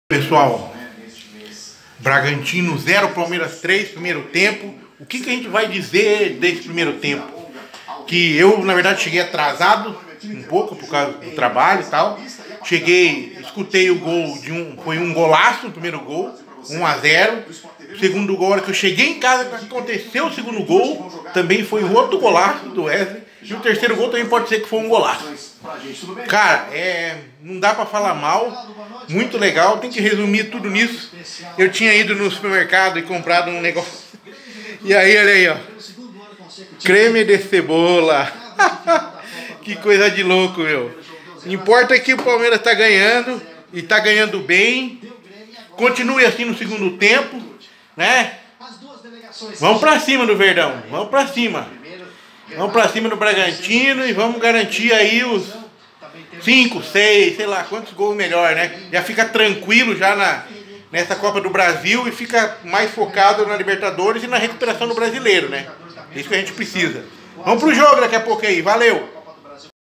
Pos Primeiro tempo Copa do Brasil 2020 Palmeiras parece outro. Red Bull Bragantino 0x3 PALMEIRAS
Palmeiras vai goleando o Bragantino fora de casa na copa do Brasil jogo de ida. 3 x 0